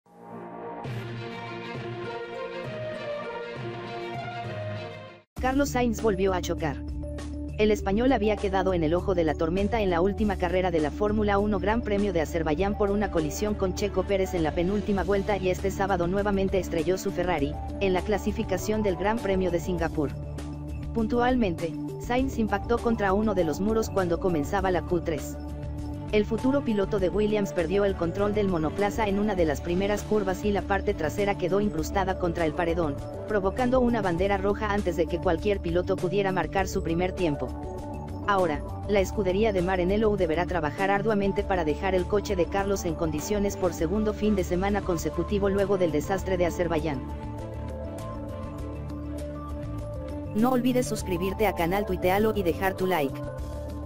Carlos Sainz se estrella en la Q3 del GP de Singapur español choca de nuevo
Carlos Sainz volvió a chocar. El español había quedado en el ojo de la tormenta en la última carrera de la Fórmula 1 Gran Premio de Azerbaiyán por una colisión con Checo Pérez en la penúltima vuelta y este sábado nuevamente estrelló su Ferrari, en la Clasificación del Gran Premio de Singapur.

Puntualmente, Sainz impactó contra uno de los muros cuando comenzaba la Q3. El futuro piloto de Williams perdió el control del monoplaza en una de las primeras curvas y la parte trasera quedó incrustada contra el paredón, provocando una bandera roja antes de que cualquier piloto pudiera marcar su primer tiempo.

Ahora, la escudería de Maranello deberá trabajar arduamente para dejar el coche de Carlos en condiciones por segundo fin de semana consecutivo luego del desastre de Azerbaiyán.